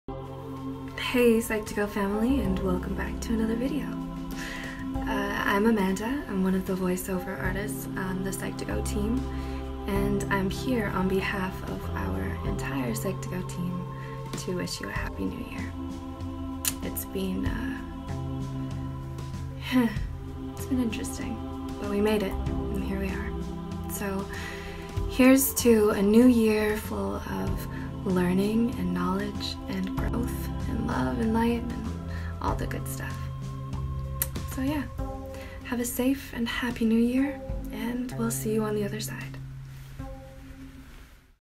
Happy New Year from Psych2Go
Happy New Year! On behalf of Psych2Go, our team want to wish you all a Happy New Year. May 2021 bring you more happiness and stronger mental wellbeing. For 2021, we will do our best to make our content more accessible around the world by translating most of our content. Hopefully, each video can help those who struggling with mental health.